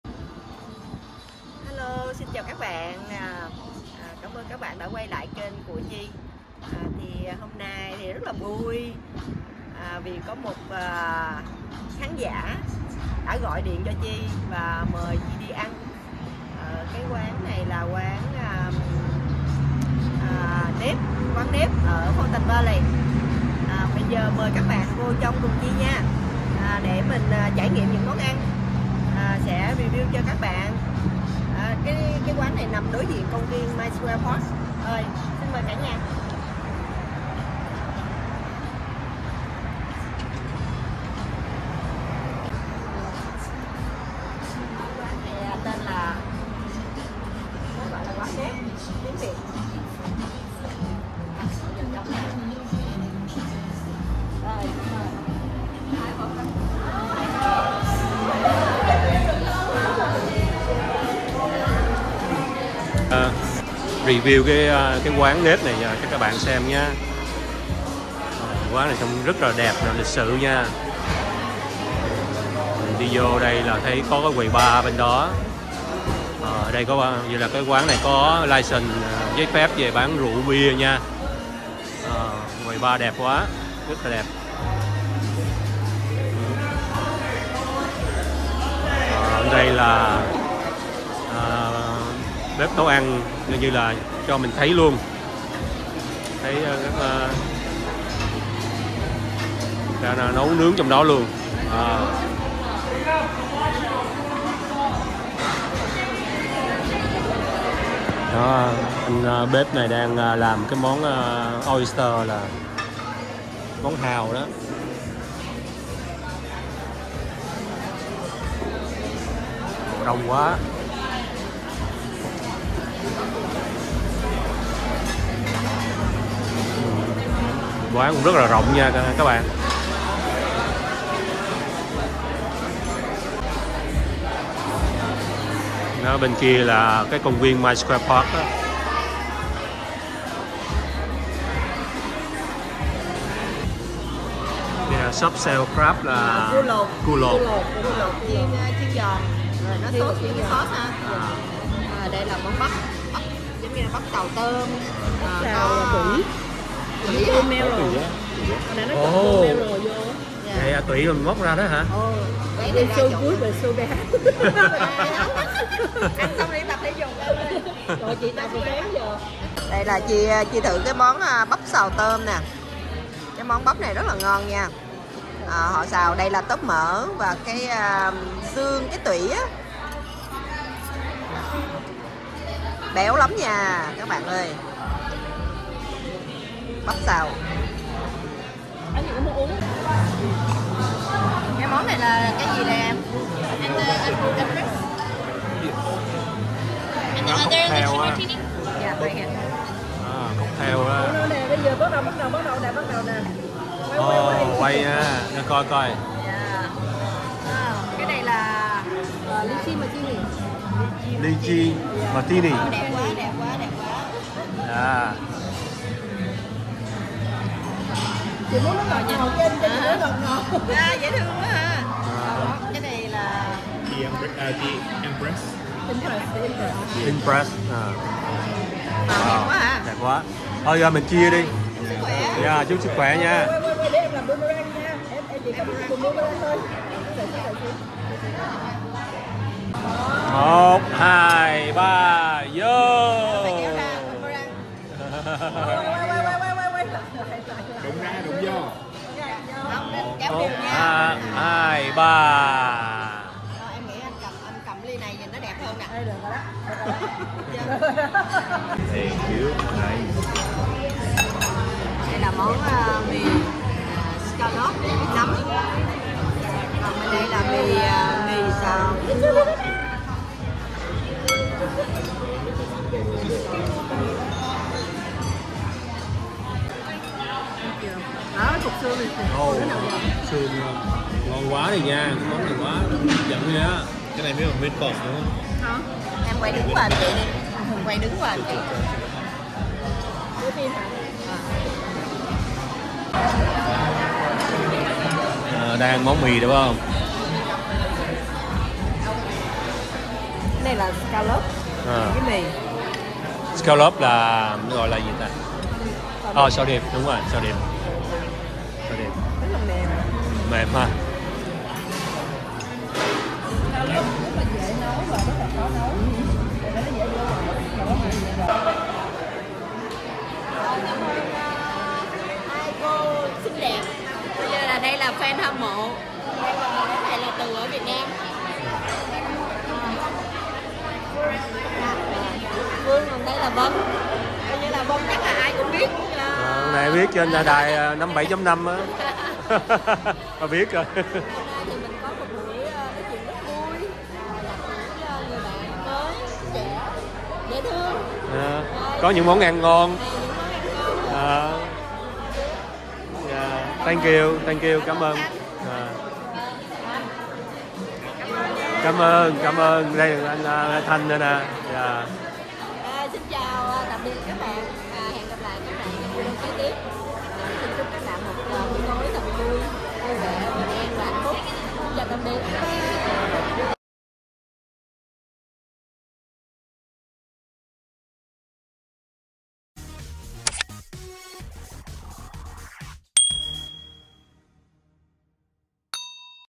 Ăn tối cuối tuần cùng bạn bè tại Gem Dining - nhà hàng với nhiều món ăn ngon, khung cảnh đẹp…vui vui
Hôm nay Chi di ăn tối cùng bạn bè tại Gem Dining - nhà hàng với nhiều món ăn ngon, khung cảnh đẹp…thật vui
Địa chỉ: 10836 Warner Ave
Chúc cả nhà luôn vui khỏe, bình an và hạnh phúc…❤️❤